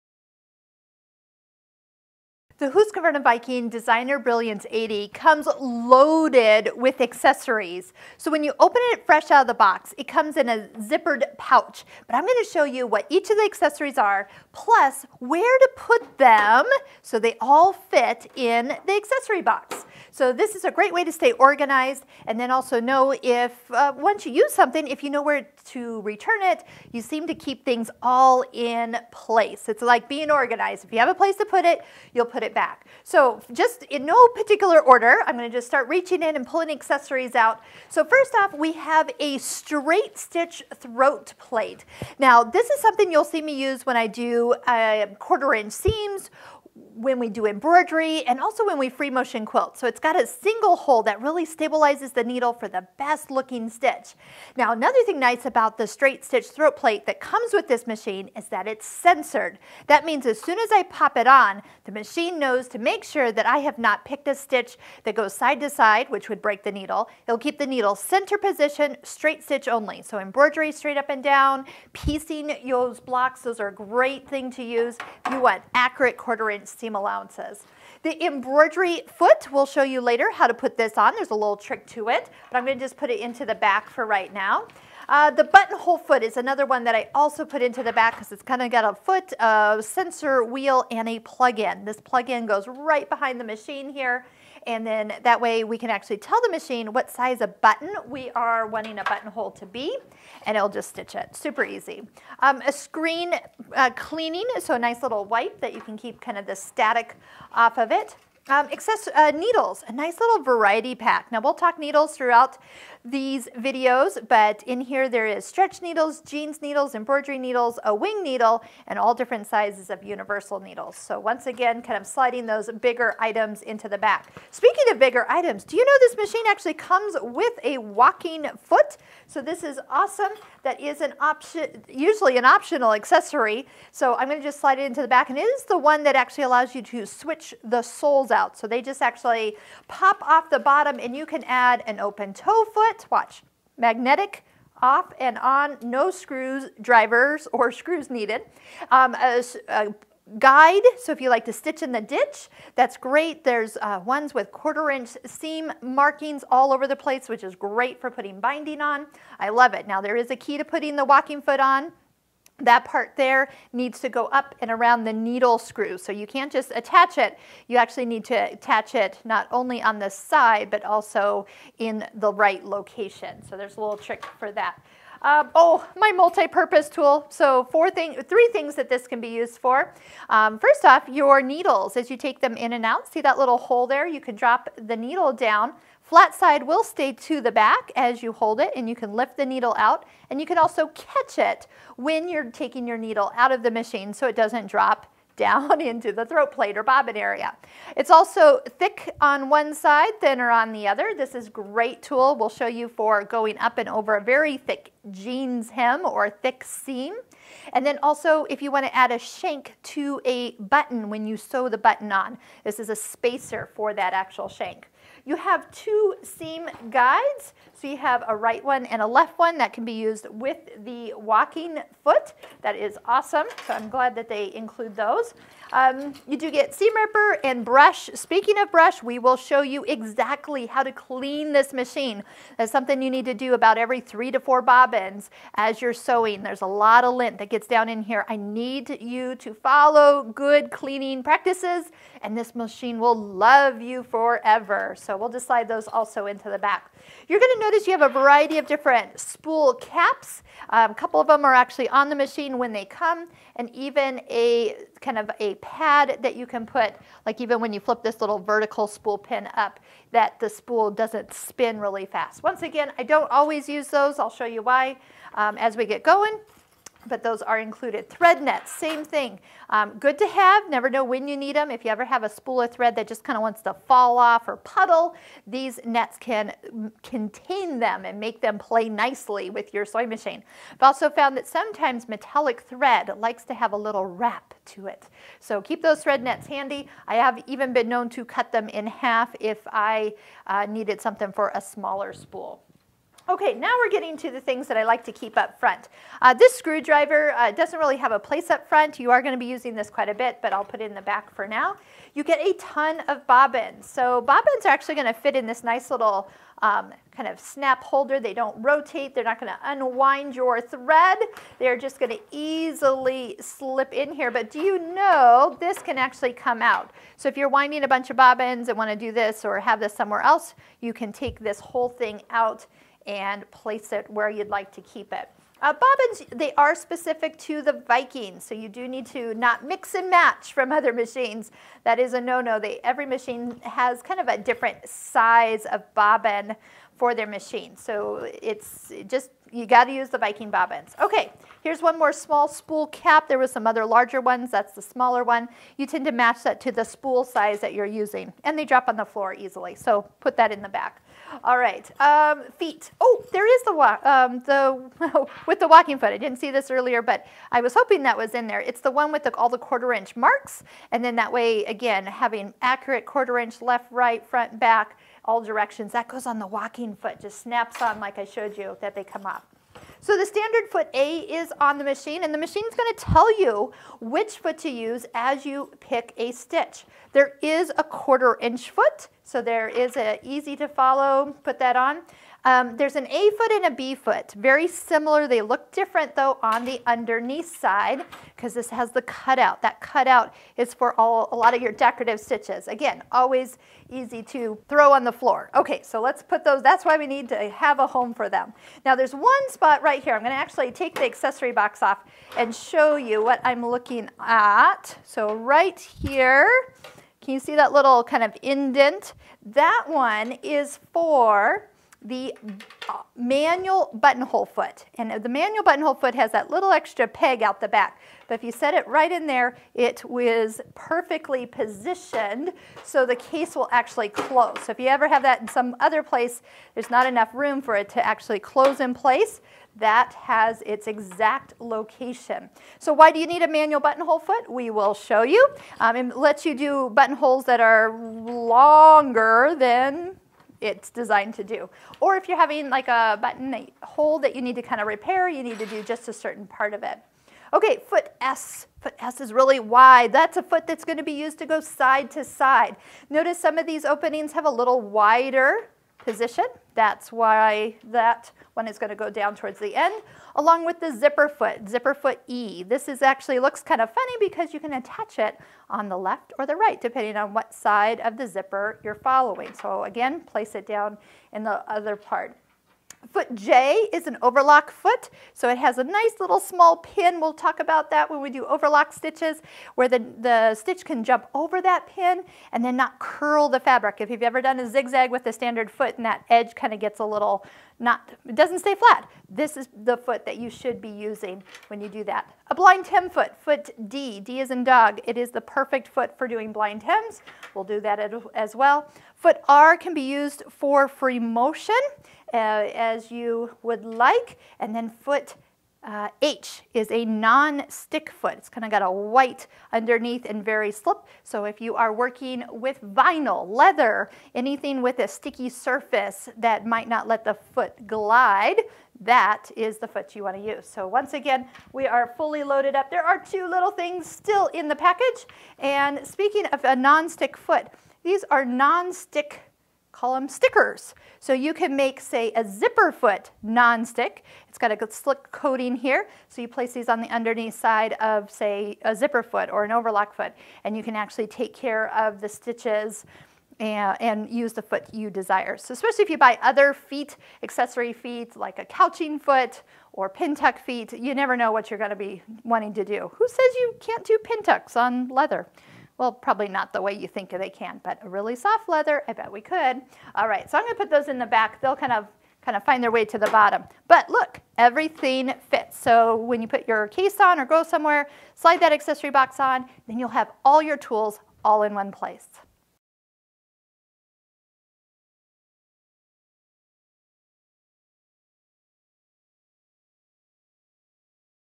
Husqvarna Viking Brilliance 80 2 Accessories
Learn which accessories come with the Husqvarna Viking Designer Brilliance 80 sewing and embroidery machine.

View all the free video tutorials on the Husqvarna Viking Designer Brilliance 80 sewing and embroidery machine: